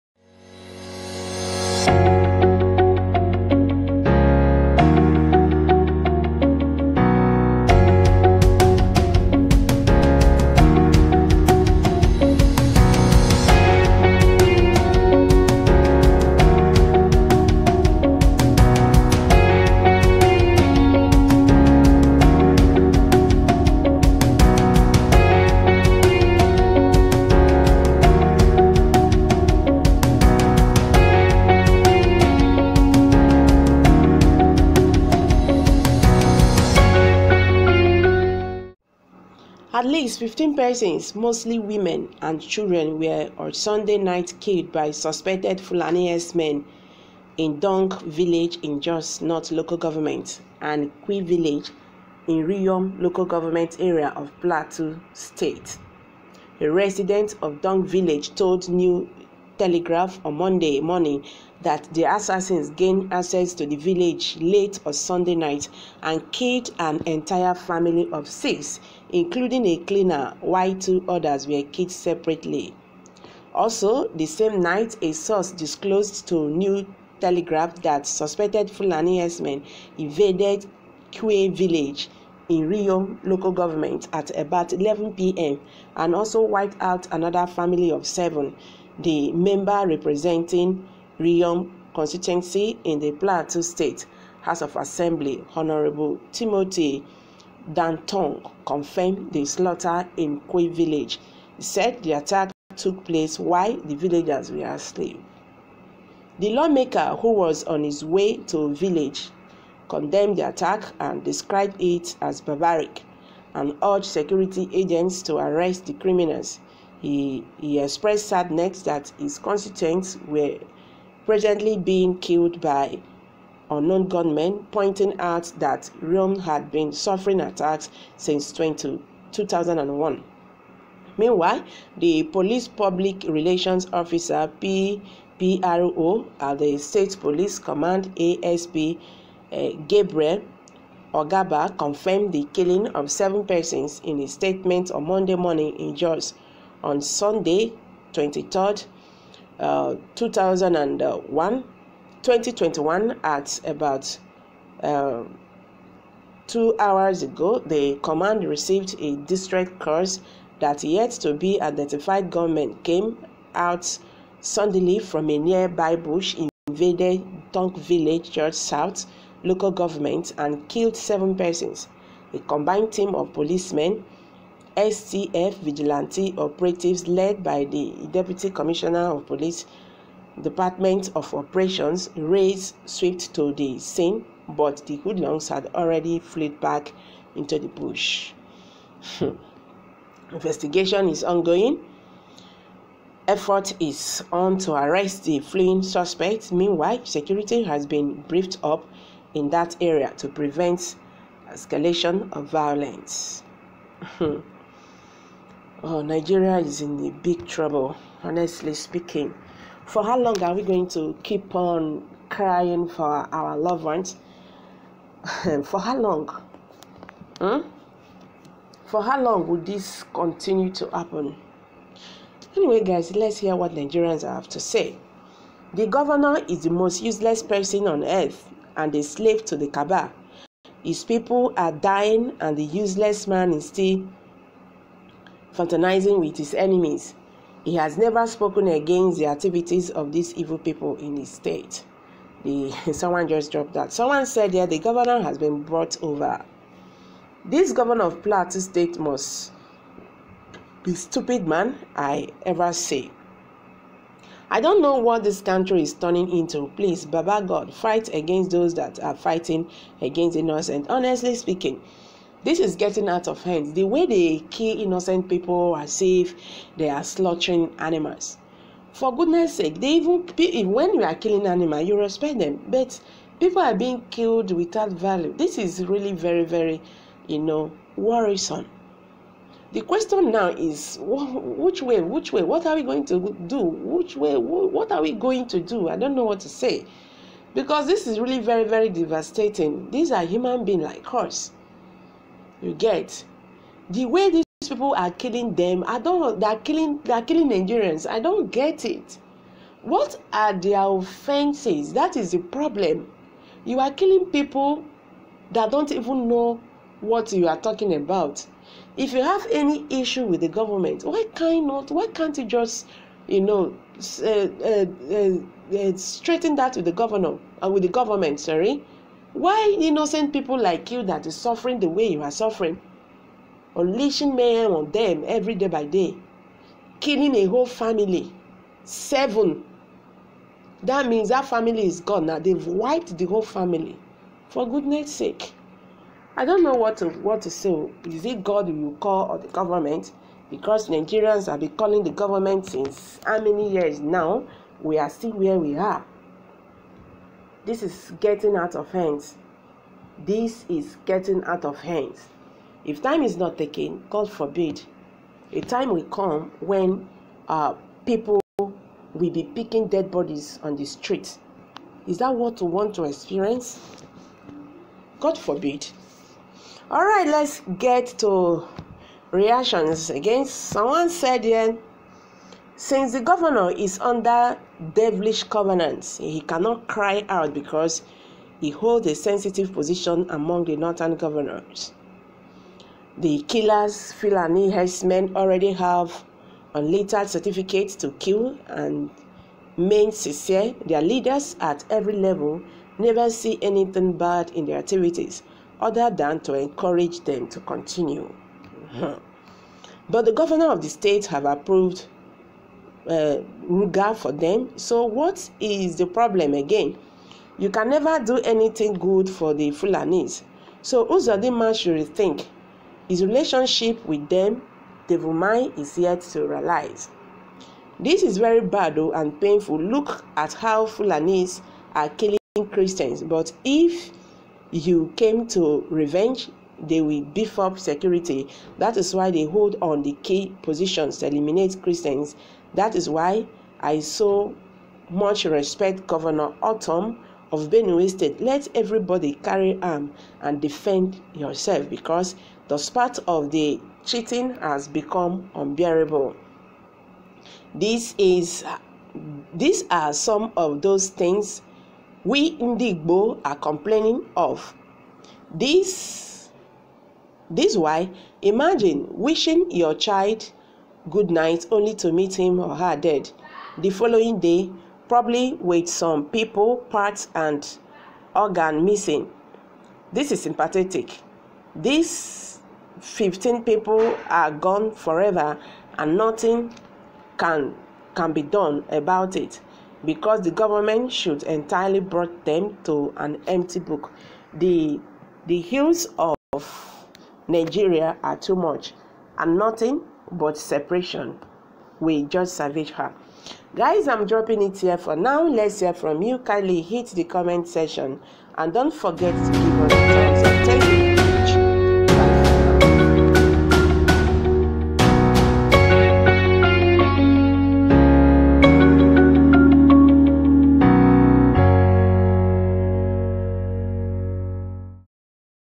TRÃG€DY: SŪSP£CT€D FULANI HERDSMEN K1LL 15 PEOPLE IN ANOTHER D€@D!¥ ATTAÇK.. REALLY SÃD!!
Fulani Herdsmen